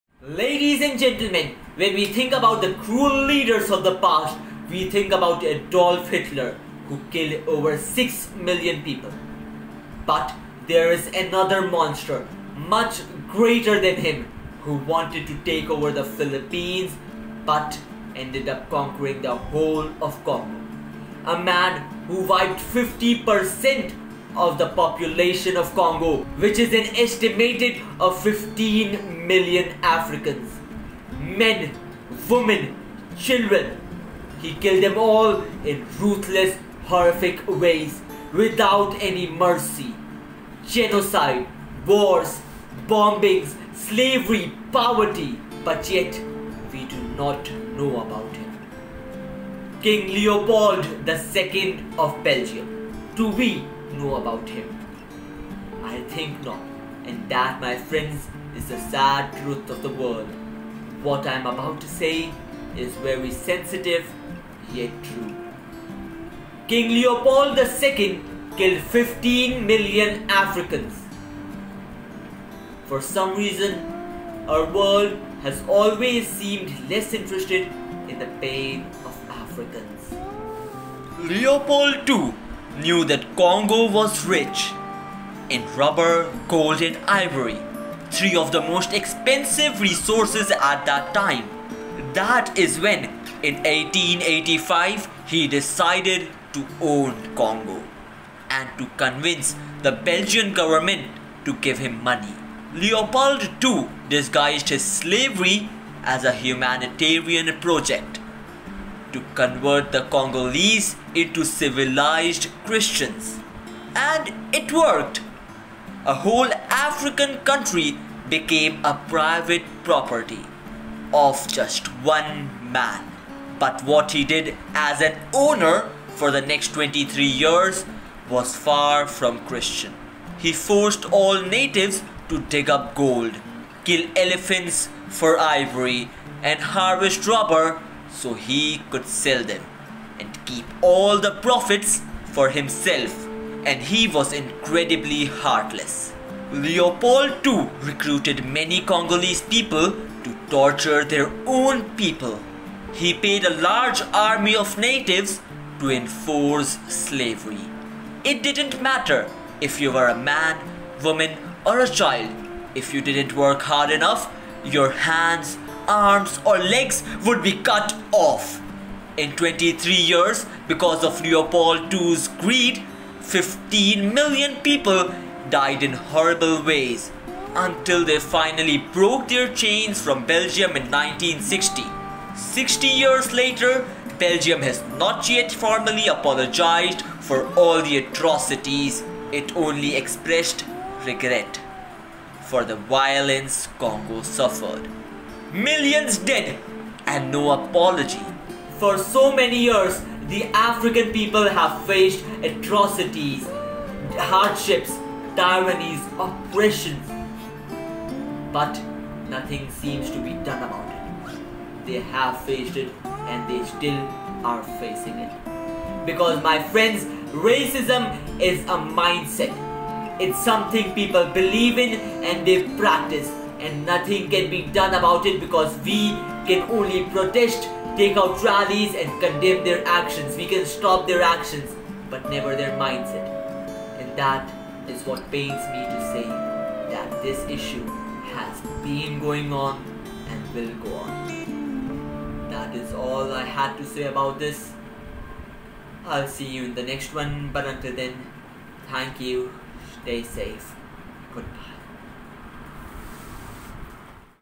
TIME TO END RACISM I KING LEOPOLD II OF BELGIUM I
I WANTED TO SPEAK ABOUT THIS MATTER FOR A LONG TIME BUT WORDS WONT SIMPLY COME OUT OF MY MOUTH. WE NEED TO END RACISM, BUT THE TRUTH IS. WE CANT. WE CANT CHANGE SOMEONE'S MINDEST, THEIR BELIEFS.